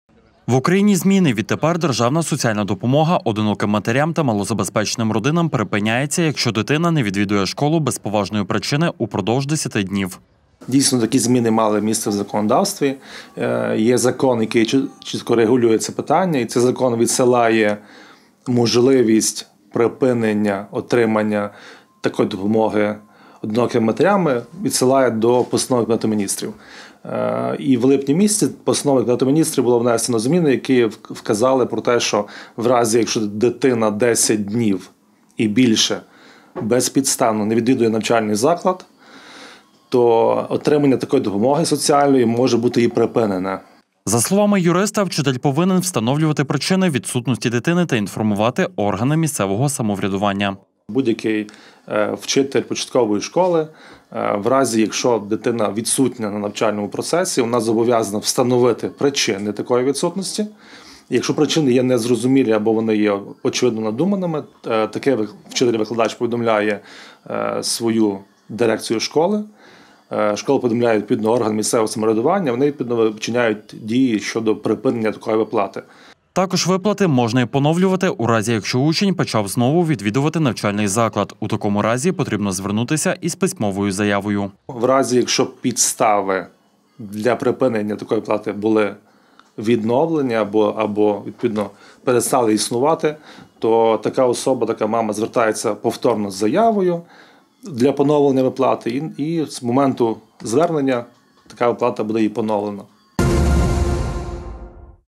Залишаться без виплат. Соціальна допомога одиноким матерям
В Україні не виплачуватимуть соціальну допомогу одиноким матерям та малозабезпеченим родинам у разі якщо дитина без поважної причини не відвідуватиме навчальний заклад більше 10 днів. В Україні зміни. Відтепер державна соціальна допомога одиноким матерям та малозабезпеченим родинам припиняється, якщо дитина  не відвідує школу без поважної причини 10 днів. За словами юриста вчитель повинен встановлювати причини відсутності дитини та інформувати органи місцевого самоврядування.Також виплати можна і поновлювати у разі, якщо учень почав знову відвідувати навчальний заклад, у такому разі потрібно звернутися із письмовою заявою.